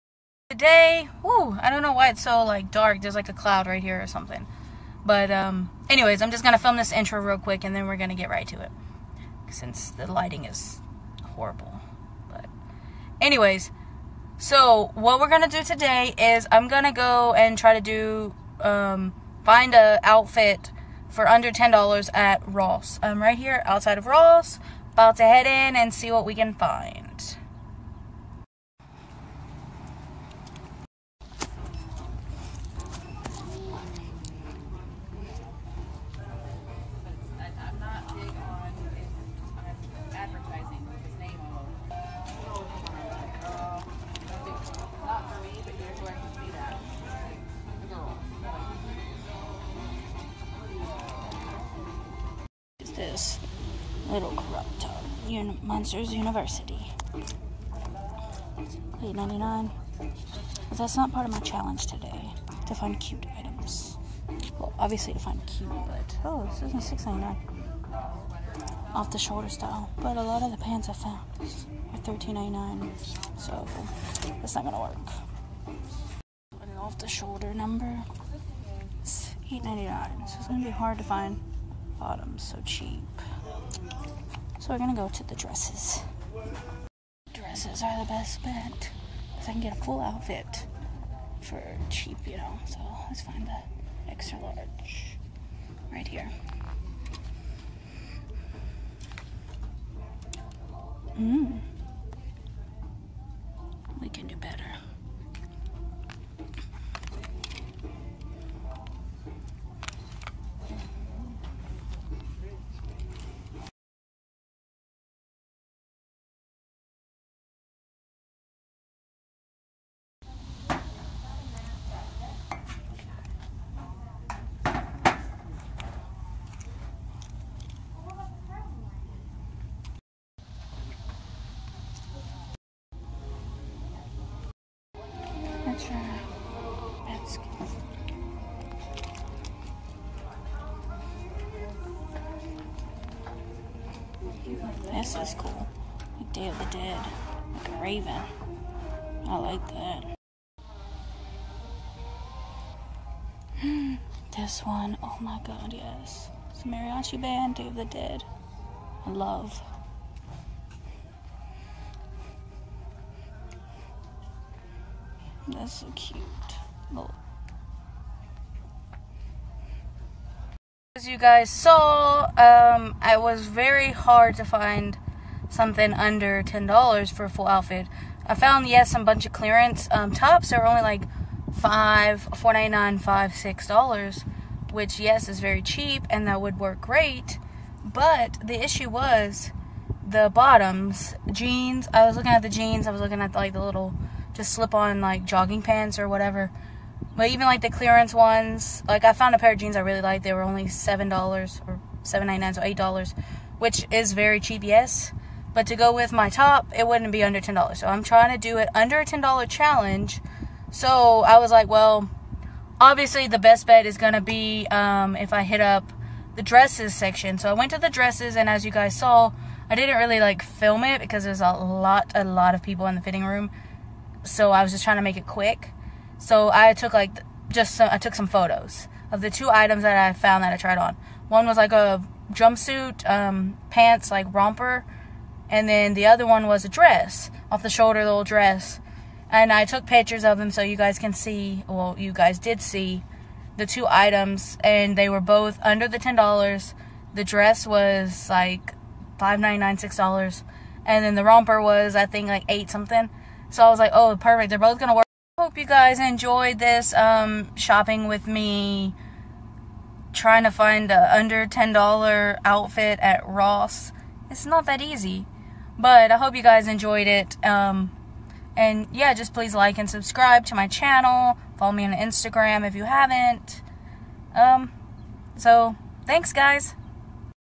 Shopping Challenge at Ross.
Going shopping at ross for a ten dollar outfit. Lets see how it goes. 😵😮